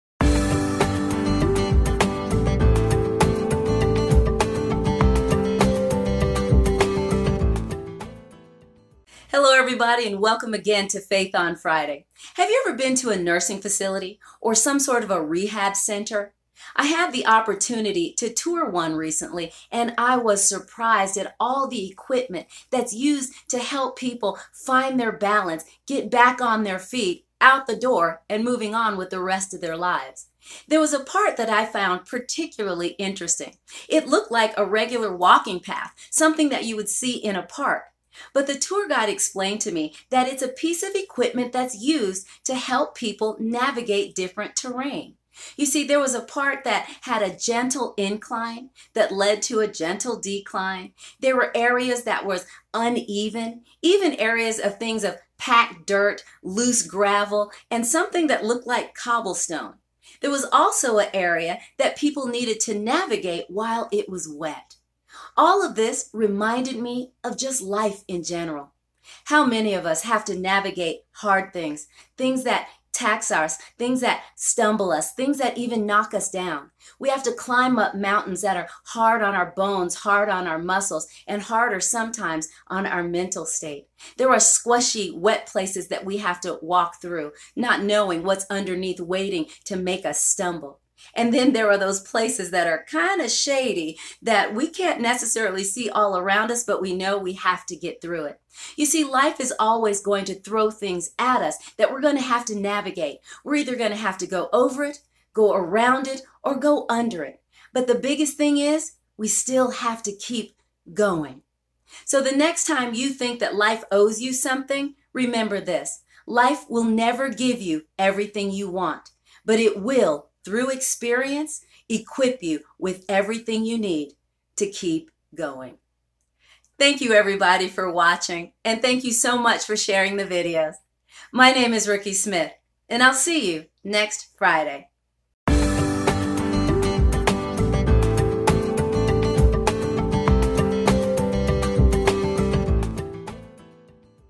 Faith on Friday - Walk the Path
In life there will be  different kinds of ground (terrain) we will have to navigate: hard, hilly, soft, squishy, wet, and shady. It's not always going to be easy, but it will be possible. Your life experiences have prepared you for whatever path you will come across. Walk with confidence.